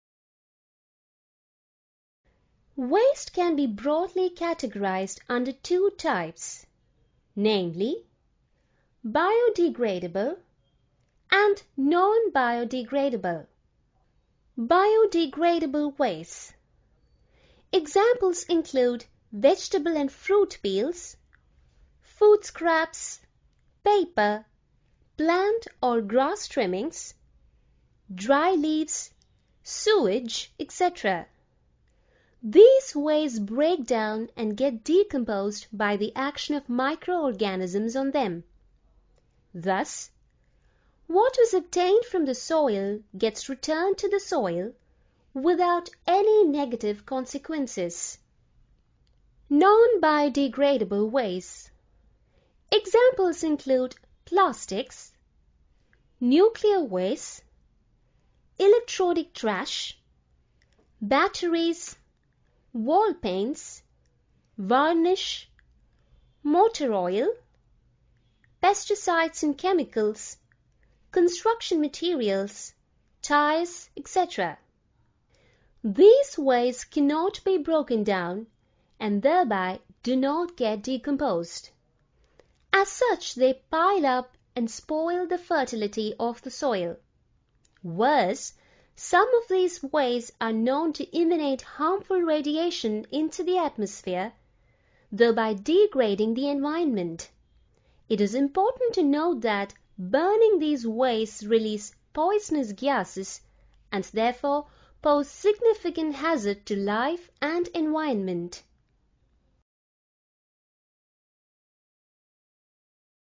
Biodegradable and Non-Biodegradable Waste
Biodegradable waste is a type of waste, typically originating from plant or animal sources, which may be degraded by other living organisms.Waste which cannot be decomposed by biological process is known as Non-biodegradable wastes.